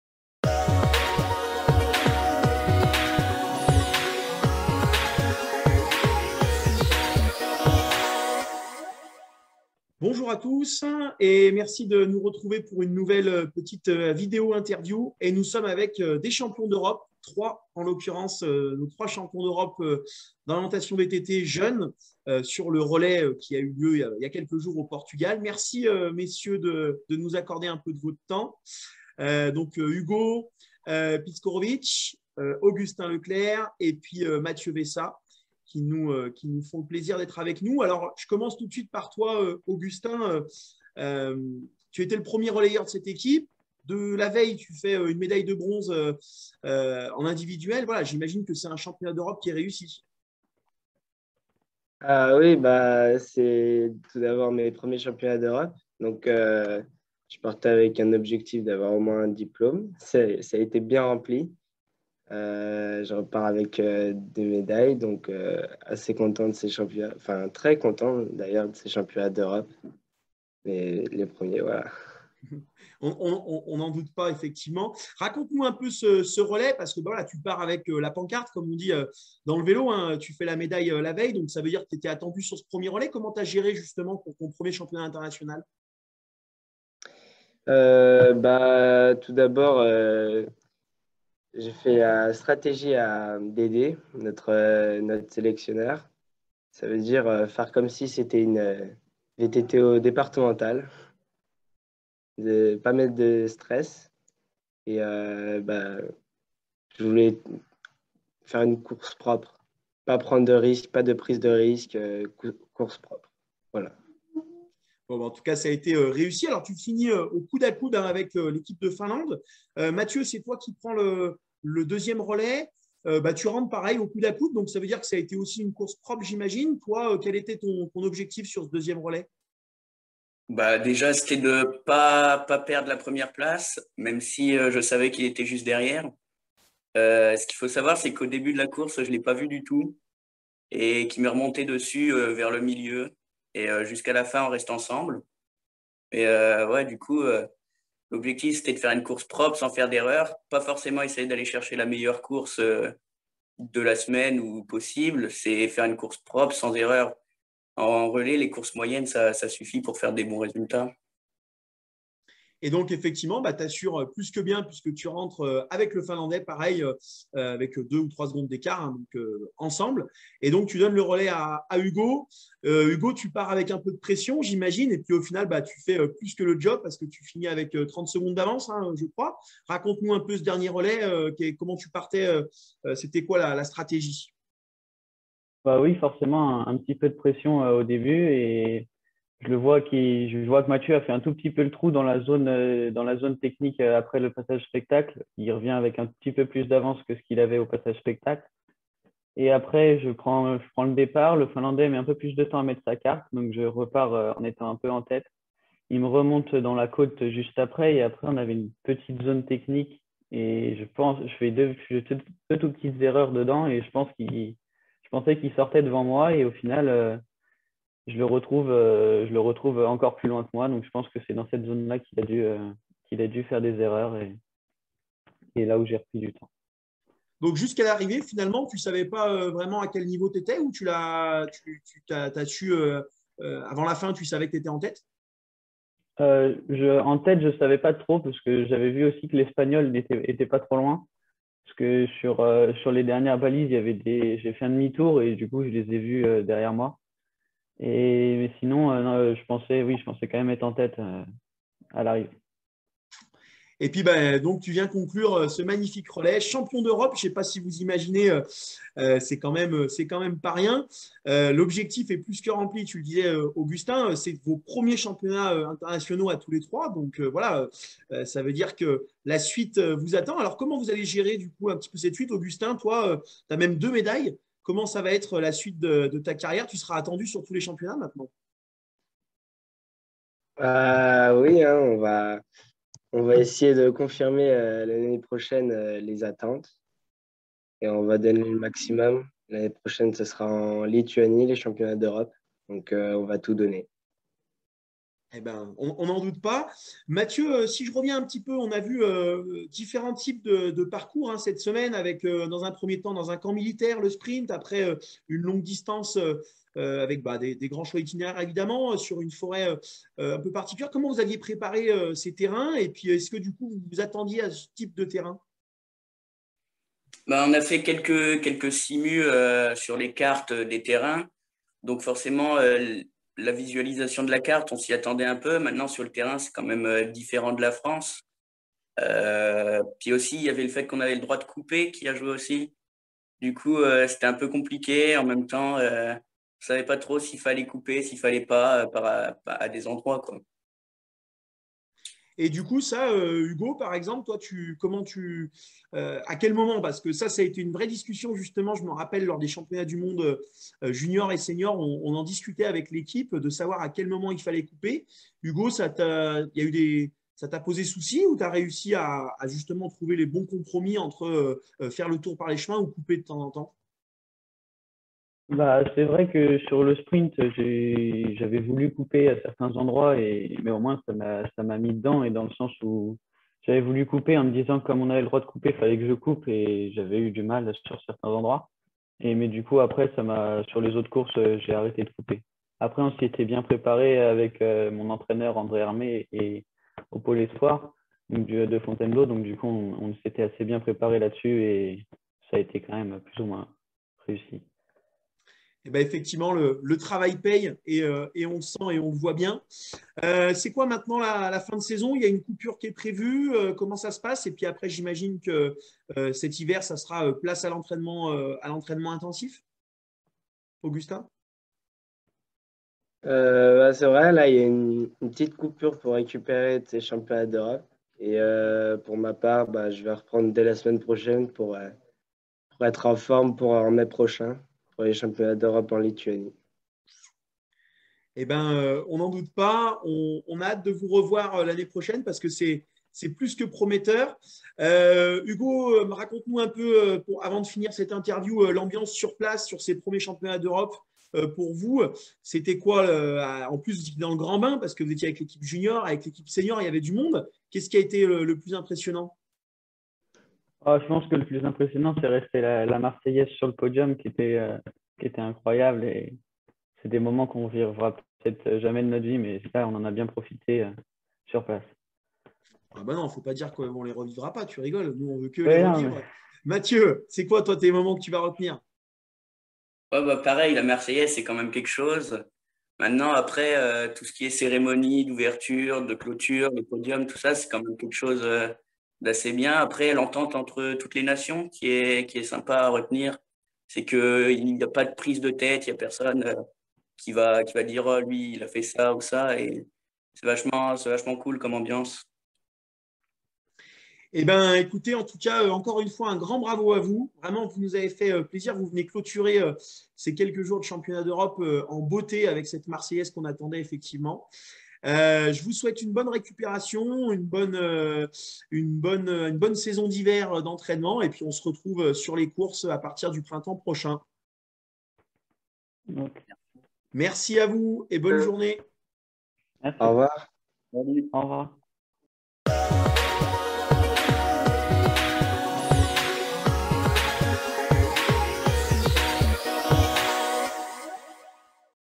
interview des champions d'Europe de relais jeune d'orientation à VTT
Augustin Leclere, Mathieu Vayssat et Hugo Piszczorowicz, reviennent pour nous sur le relais victorieux des championnats d'Europe jeune d'orientation à VTT au Portugal.